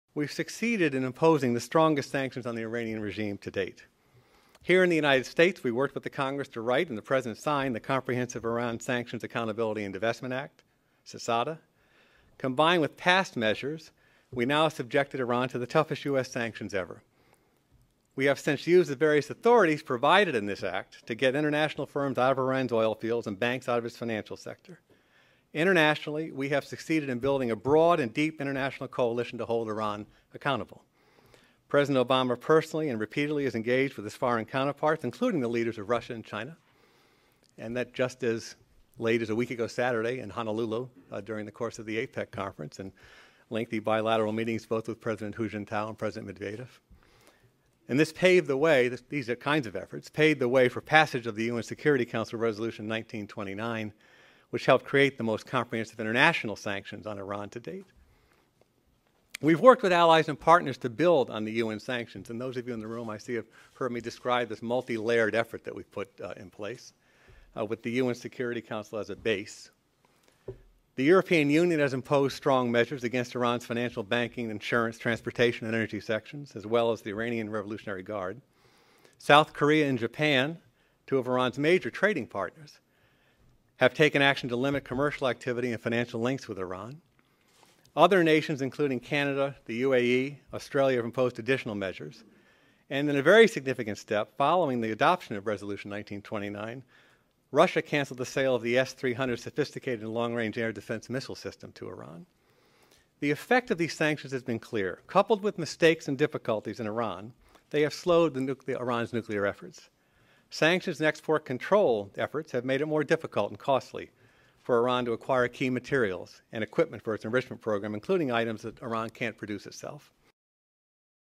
Strongest Sanctions On Iran to Date
Tom Donilon, National Security Advisor, The White House: Working with Congress and the international community, the United States has succeeded in imposing the strongest sanctions on Iranian regime to date resulting in slowing their nuclear efforts. More